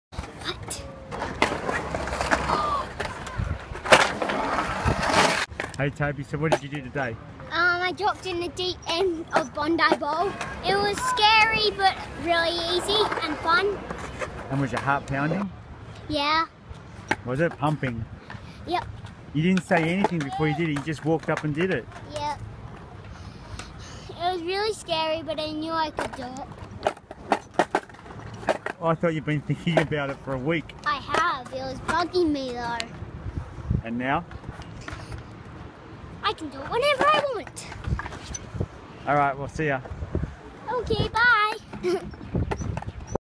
Toby Drops In Deep @ Bondi
Toby Does his first drop in at the deep end of Bondi Bowl